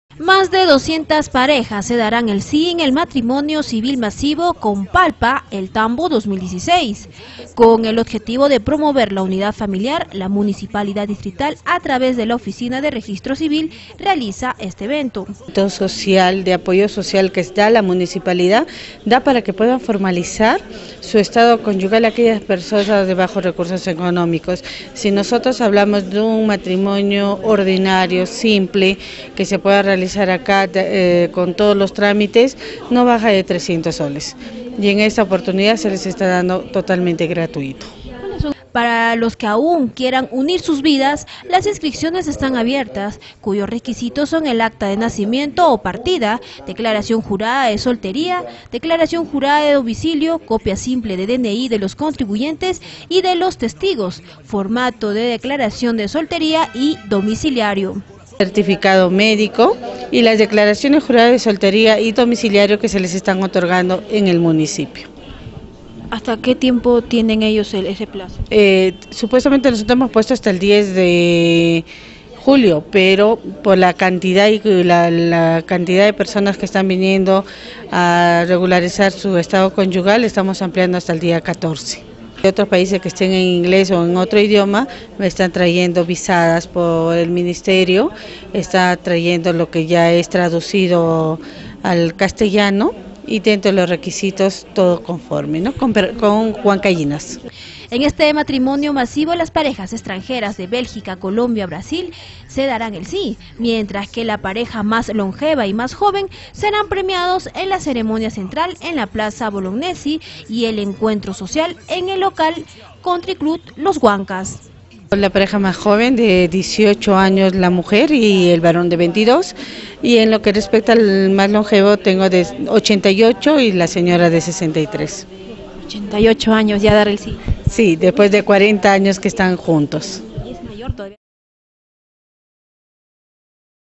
MATRIMONIO MASIVO CON PALPA CADENA NOTICIAS
Más de 200 parejas se darán el sí en el "Matrimonio Civil Masivo con Palpa El Tambo 2016"